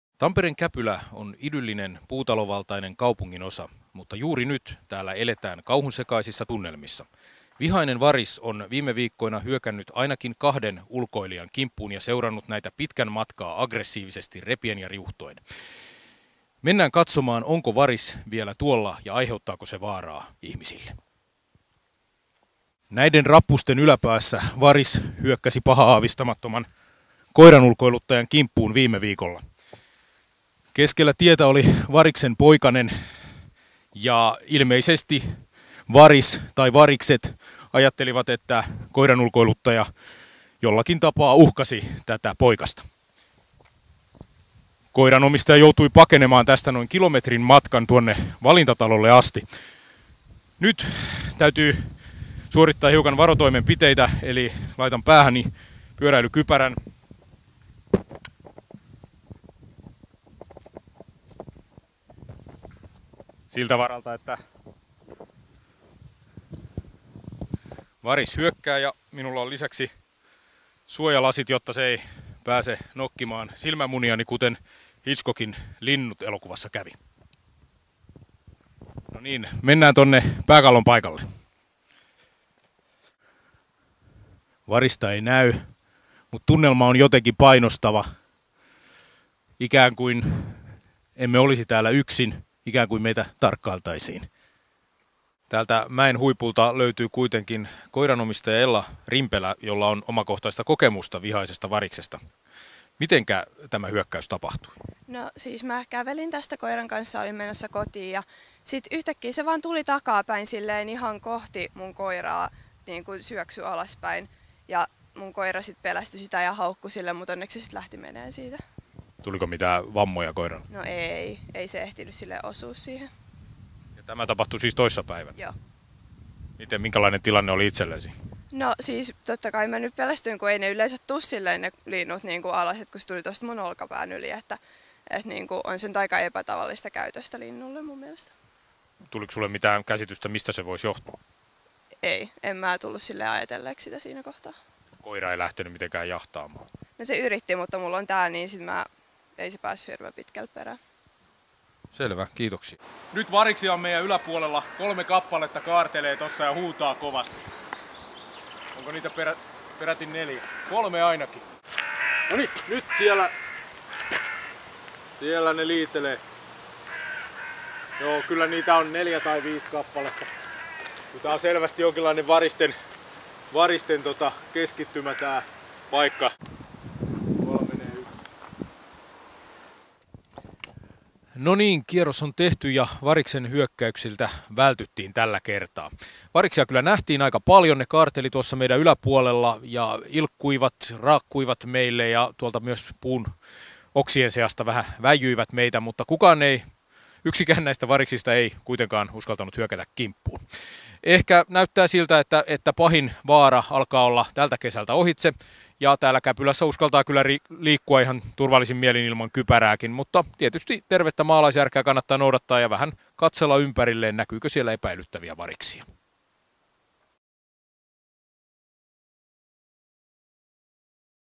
Varis Käpylässä
Varikset häiriköivät Tampereen Käpylässä. Moro vieraili paikalla, jossa varis on hyökännyt useiden ihmisten kimppuun.